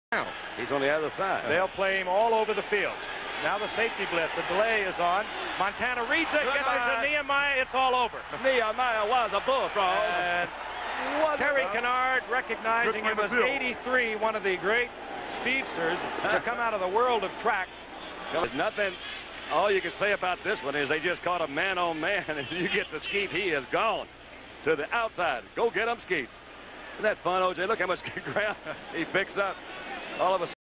Joe Montana to Renaldo Nehemiah
Joe Montana hits Renaldo Nehemiah for a touchdown in a 1984 game against the New York Giants